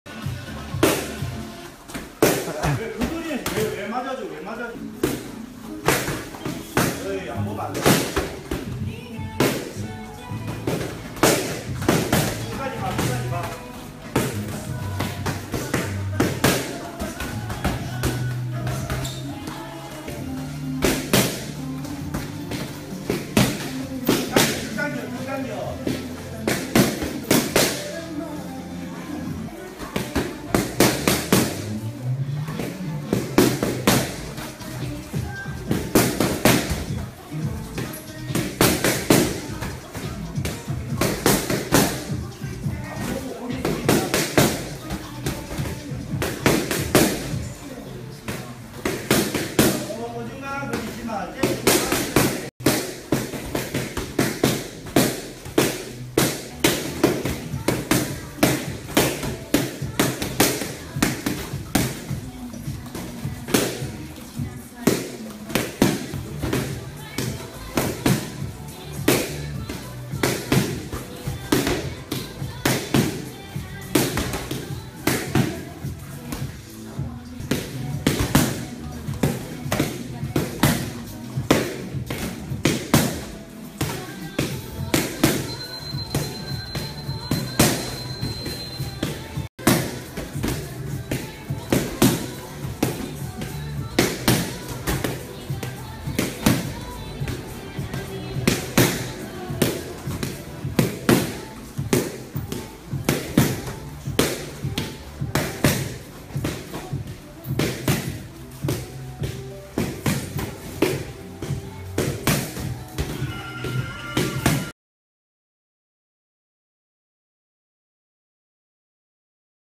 페더급 vs 플라이급 (플라이급이 페더급을 다운시키다??) 프로들의 스파링!!
실전 스파링 훈련영상 입니다.